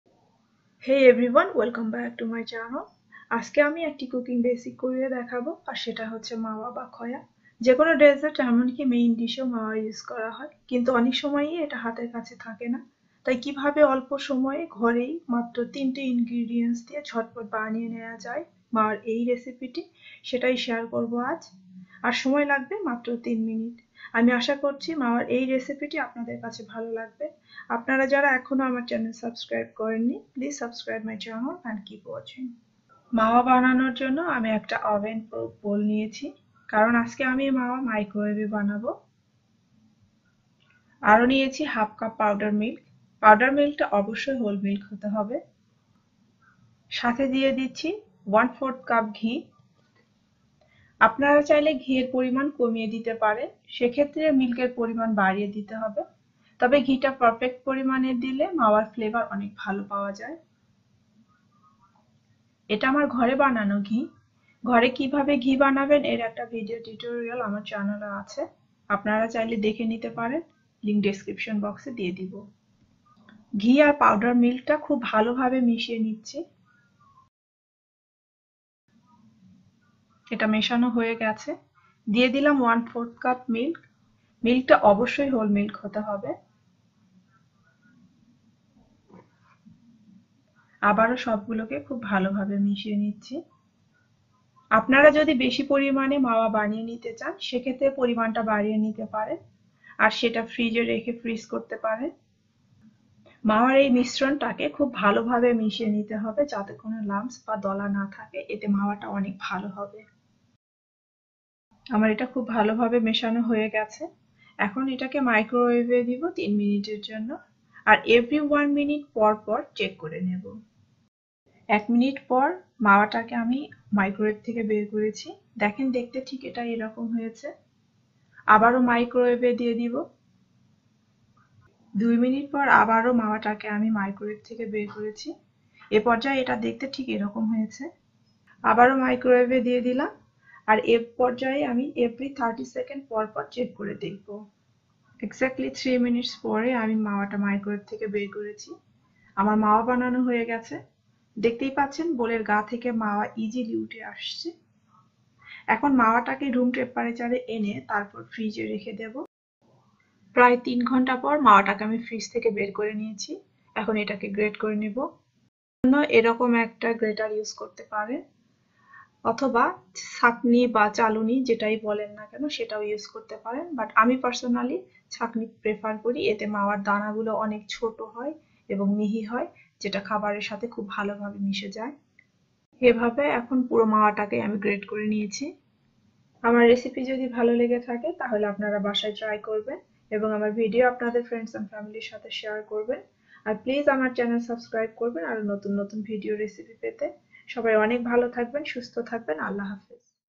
৩ মিনিটে ইনস্ট্যান্ট মাওয়া মাত্র ৩টি উপকরণ ll 3 Minutes Easy & Instant Mawa Recipe ll Episode 19
Ingredients
Powder milk 1/2 cup
ghee/butter 1/4 cup